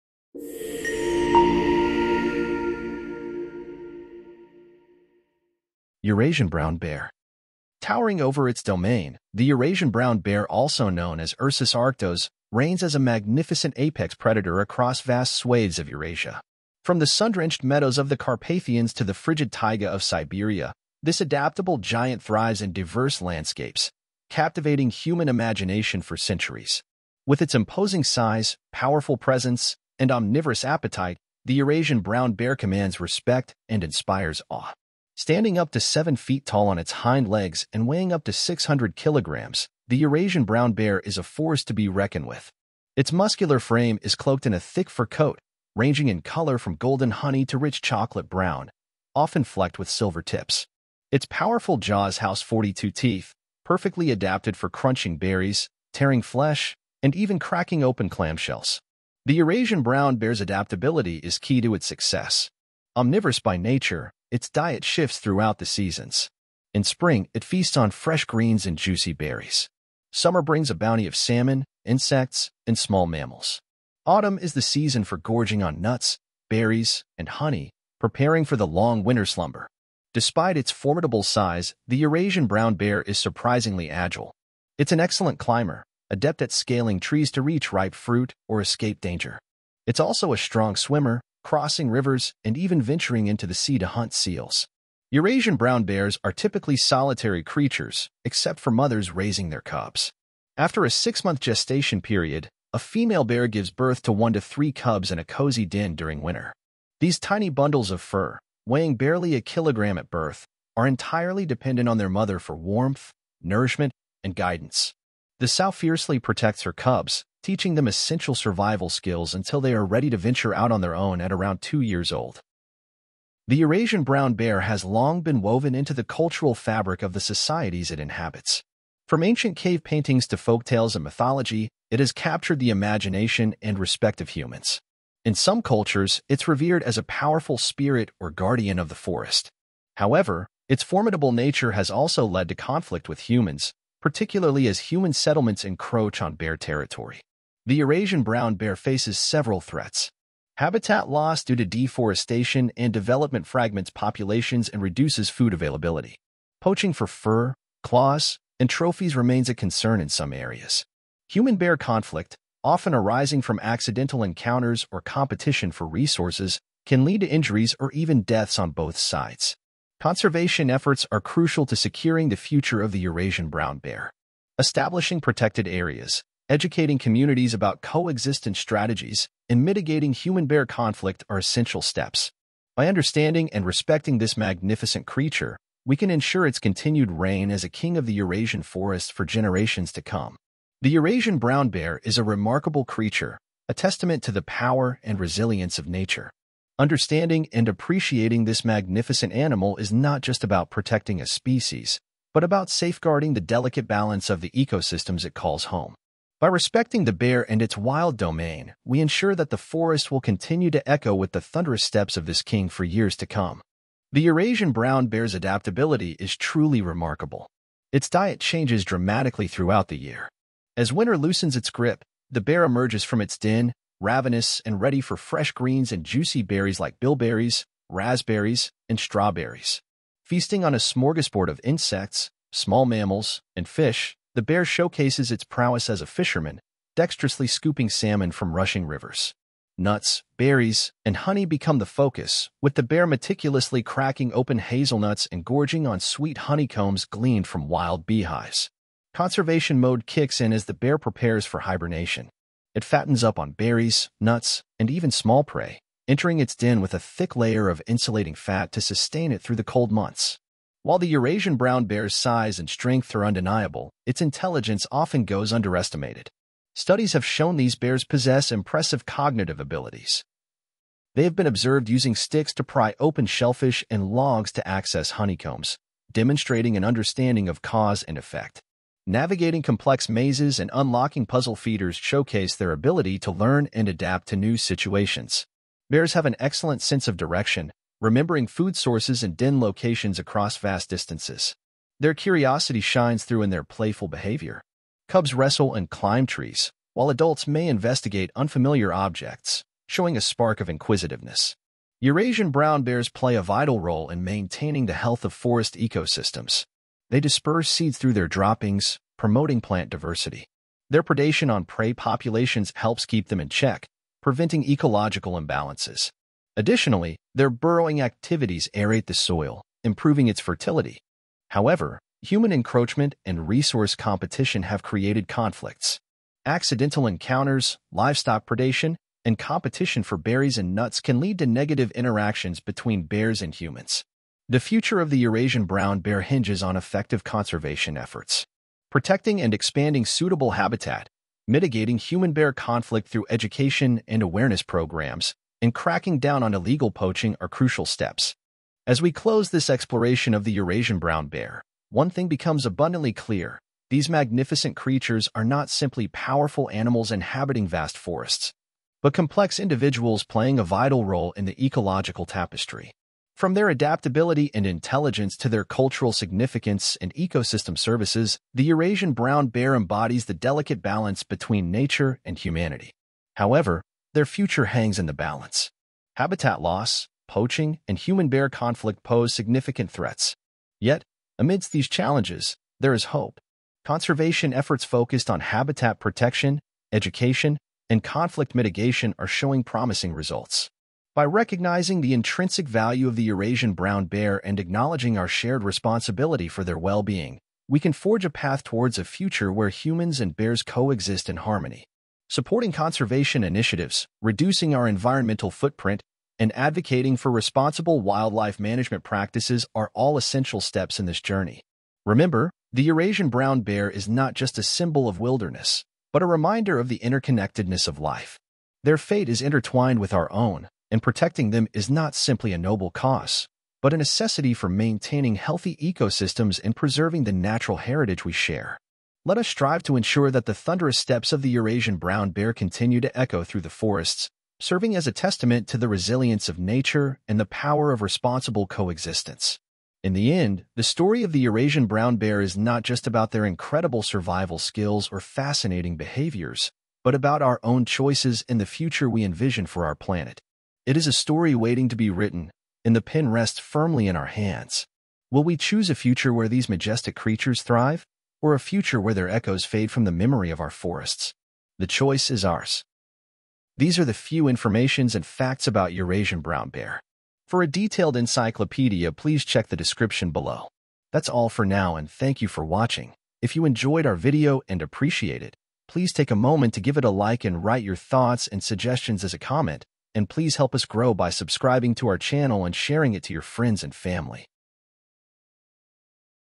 Eurasian Brown Bear - Audio / Video Encyclopedia
The Eurasian brown bear is a subspecies of the brown bear that lives in much of Europe and Asia. It is also known as the European brown bear, the common brown bear, or simply the brown bear
Some facts about the Eurasian brown bear are:

• It has a fur color that ranges from yellowish-brown to dark brown, red-brown, and almost black. Some individuals may have a white patch on their chest.
• It is one of the largest living terrestrial carnivores, with an average weight of 135–250 kg (300–550 lb) for females and 250–300 kg (550–660 lb) for males. The Siberian brown bear, a subspecies found in eastern Russia, can weigh up to 360 kg (800 lb).
• It is omnivorous and feeds on a variety of foods, such as berries, roots, shoots, small mammals, fish, insects, and carrion. It can also prey on large ungulates, such as deer, moose, and wild boar
• It is mostly solitary, except during the mating season and when females have cubs. It communicates with other bears through vocalizations, body language, and scent marking
• It hibernates in winter, usually in a den dug in the ground or under a rock. During hibernation, it does not eat, drink, urinate, or defecate, but relies on its stored fat for energy.